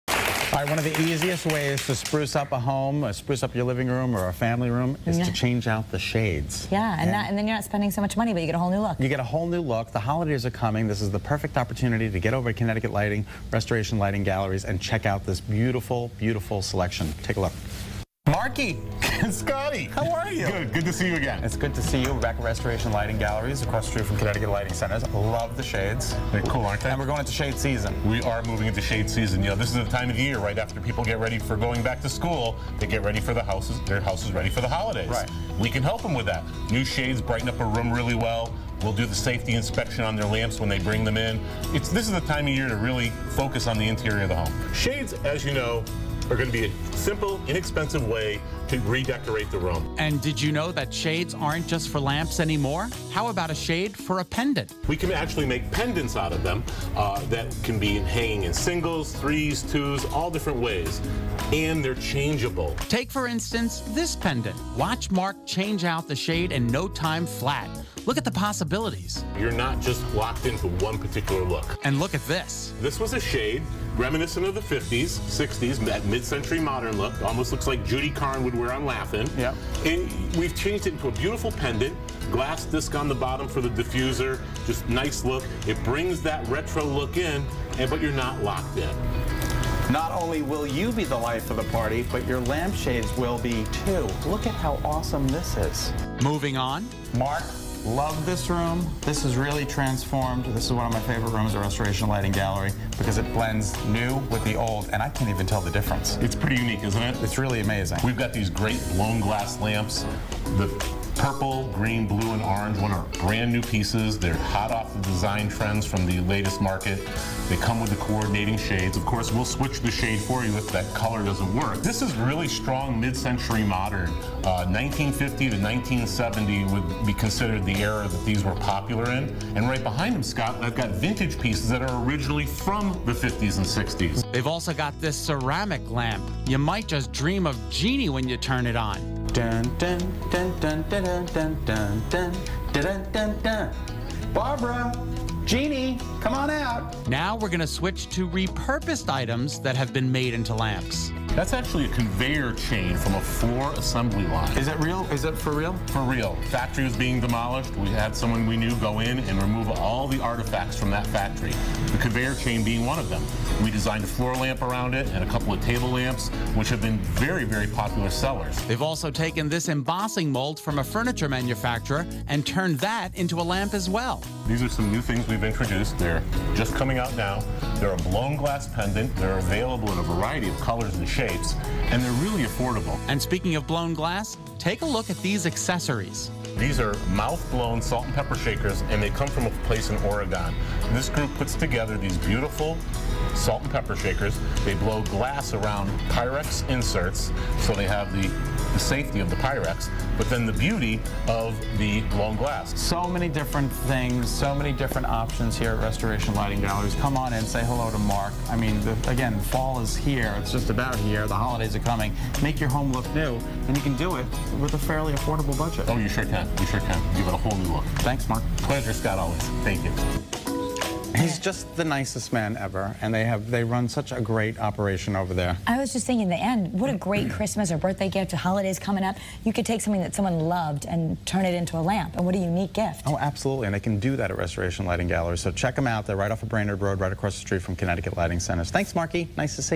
CT Lighting Centers: Beautiful Lamp Shades (BCT)
CT Lighting Centers & Restoration Lighting Gallery has a huge variety of lamp shades and lighting accessories on display. Recently Scot Haney visited with General Manager Mark Okun about the the new products available at Restoration Lighting Gallery.